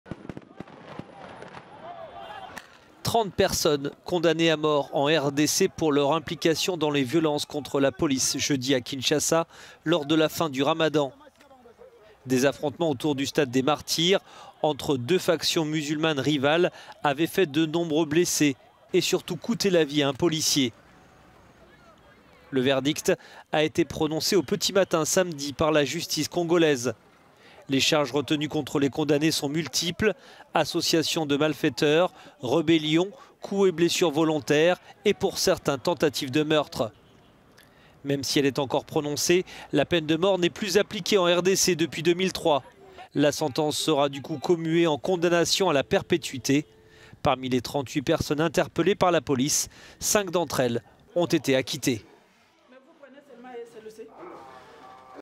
RDC : 30 condamnations à mort après les violences contre la police
Trente personnes ont été condamnées à mort en République démocratique du Congo pour leur implication dans les violences contre la police ayant marqué jeudi la fin du ramadan à Kinshasa,